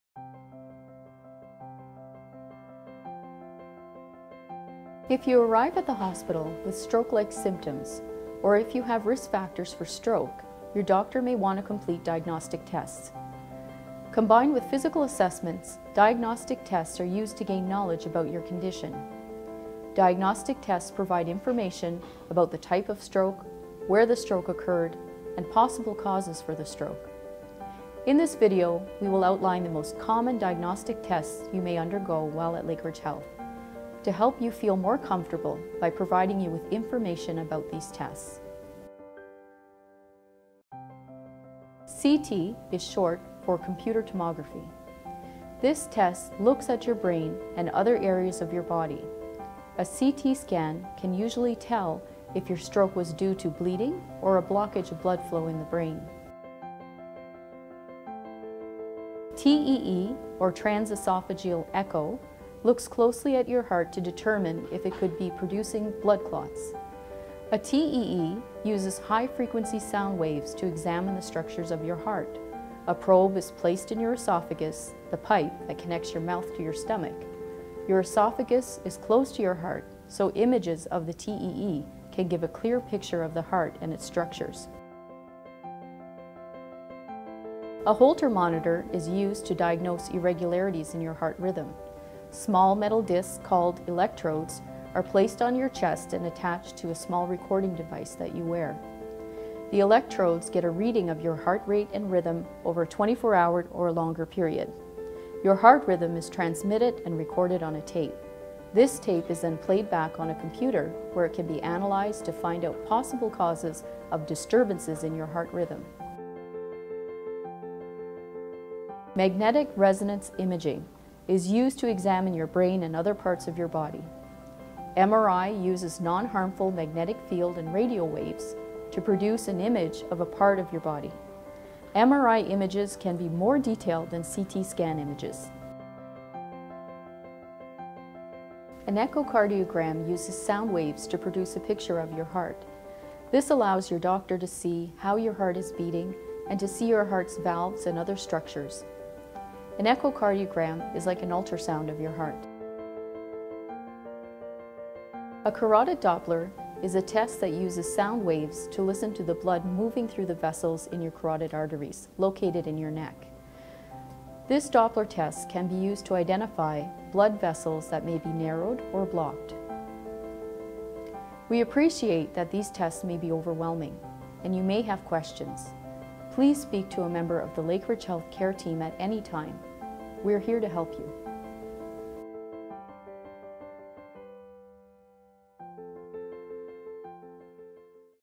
Diagnostic Testing After a Stroke
Diagnostic tests provide information about the type of stroke, where the stroke occurred and possible causes for the stroke.